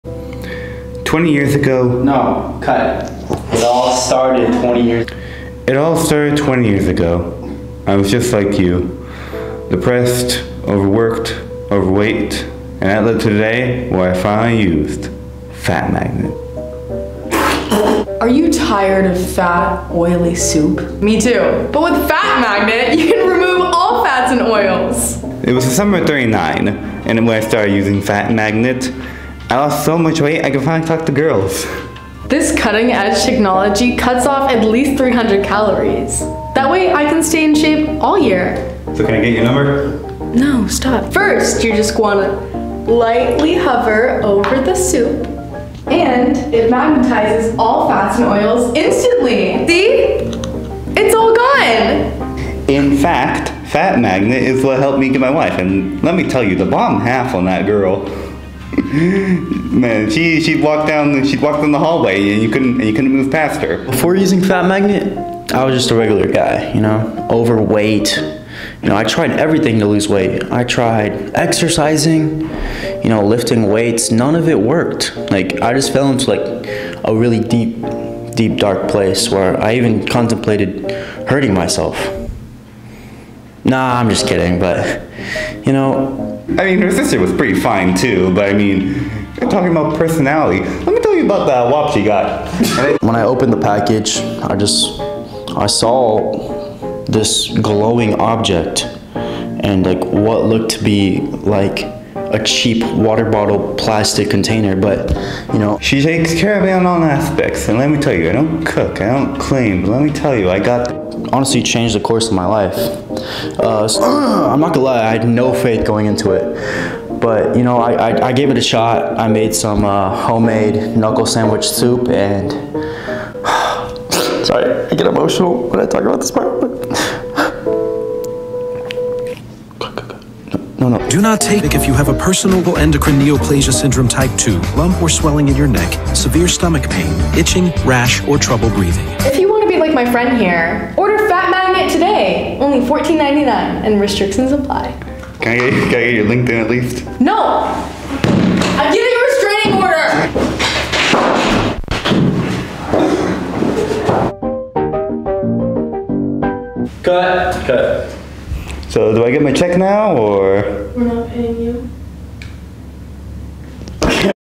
random infomercials be like...
actors: Keara Fellers and Adam Barraclough

not a real product. fat magnet commercial with our own twist. changed our lives faster than working out and exercising ever could.